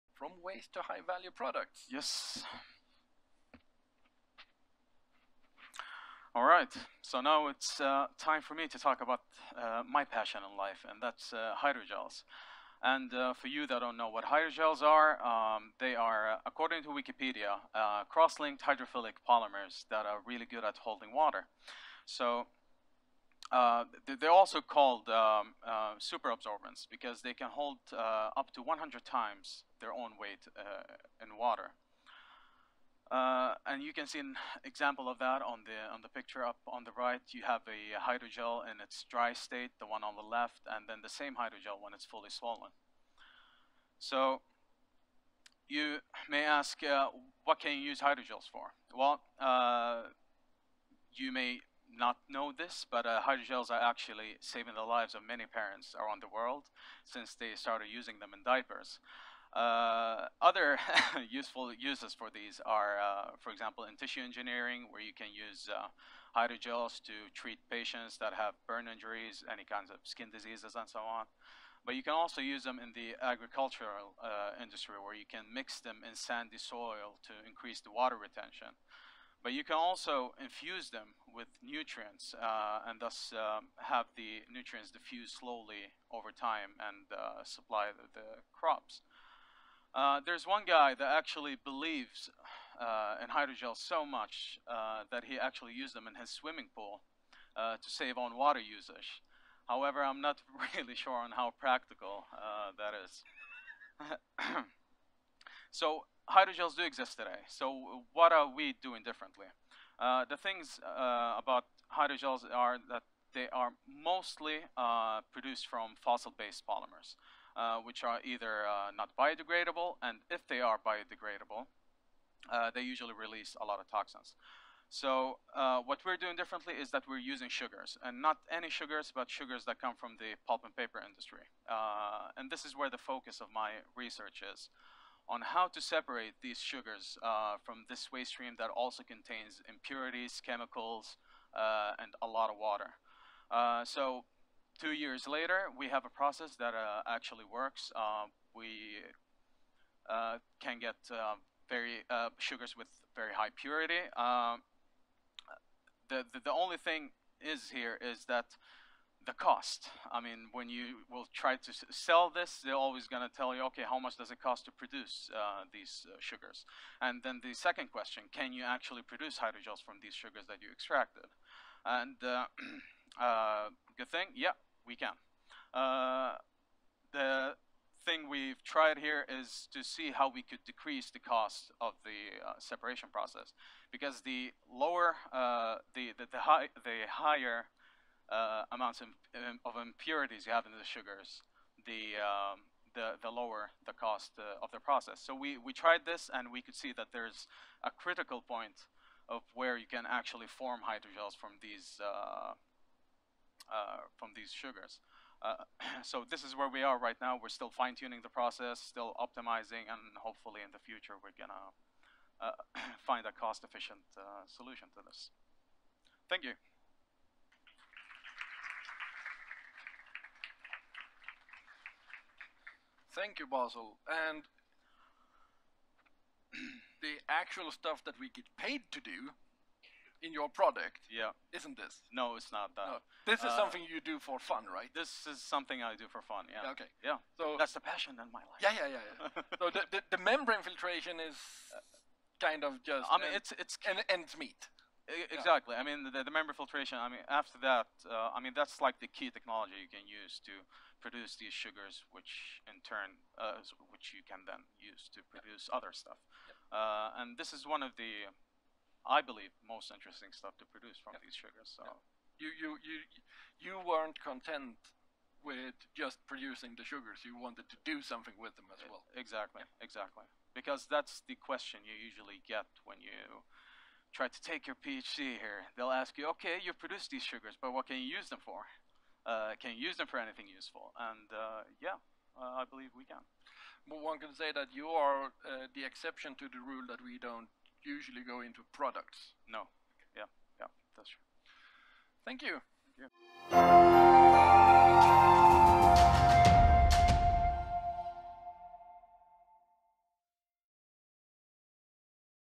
From waste to high value products: Production of hemicellulose hydrogels
Science slam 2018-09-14 by Basel Al-Rudainy